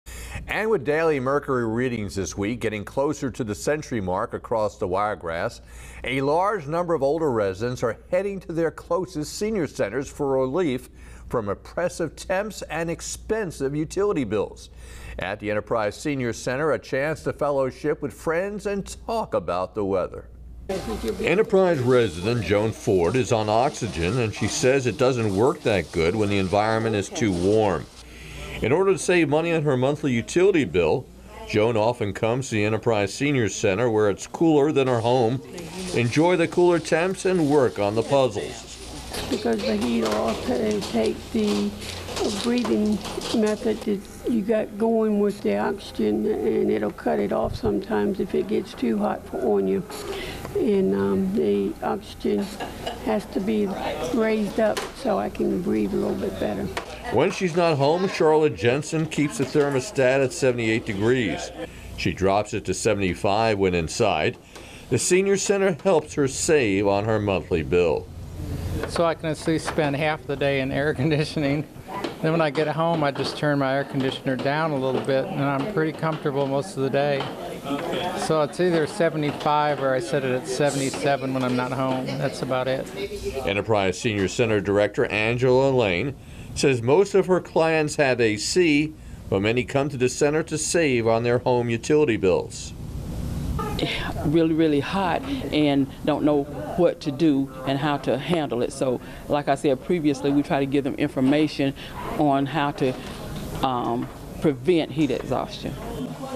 Enterprise senior citizens beating the heat
Enterprise resident Joan Ford is on oxygen, and she says it doesn't work that well when the environment is too warm. For more News, Weather, and Sports in the Wiregrass